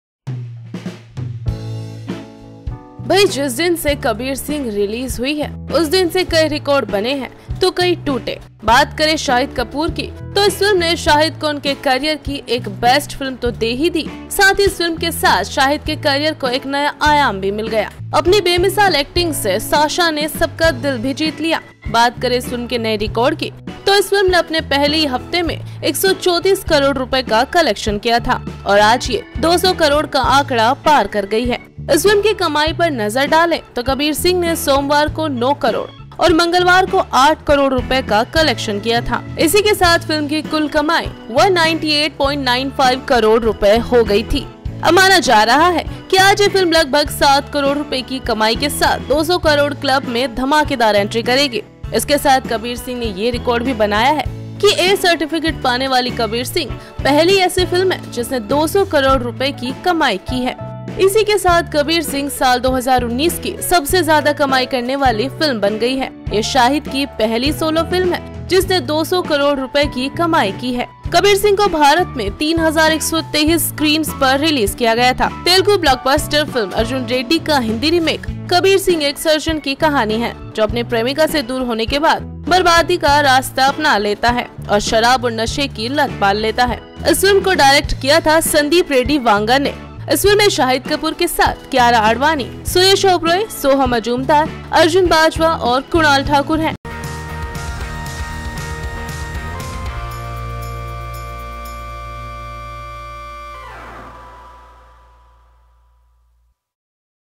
Shahid kapoor और Kabir singh दोनों ने बनाया ये बड़ा रिकॉर्ड !
अपनी बेमिसाल एक्टिंग से साशा ने सबका दिल जीत लिया बात करे इस फिल्म के नए रिकॉर्ड की तो इस फिल्म ने अपने पहले हफ्ते में 134.42 करोड़ रुपये का कलेक्शन किया था और आज ये dekhe video